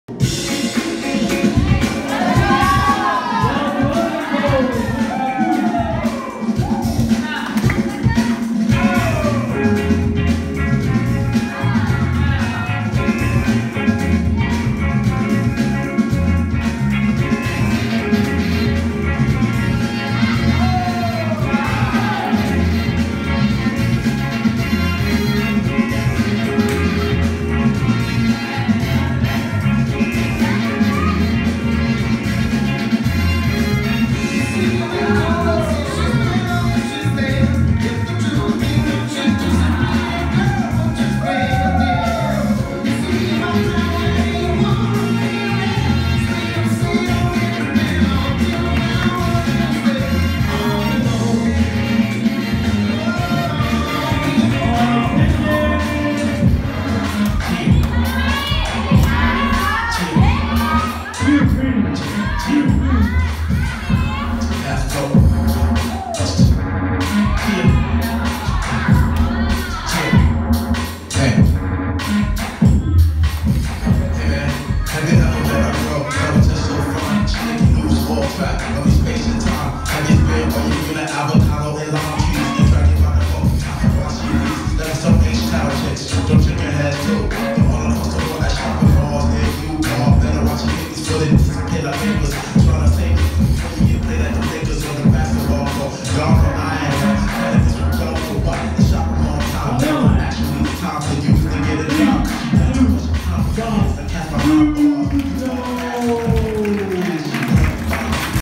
むらたく(L) VS YUMI(HH) | Best 16-5 | CHECK MY FLAVA vol.33 2019.09.07｜CHAMPION 3rd season
●JUDGE
・TAKAO(DECADE DIFFERENCE)
・克哉(Buccaneer crew)
・TORA(ZOOOTY,Owlurk)

◆MC
KFIA

◆BATTLE・FLOOR DJ
BLYS

-Movie-
CHECK MY FLAVA

CHECK MY FLAVA vol.33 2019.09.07｜CHAMPION 3rd season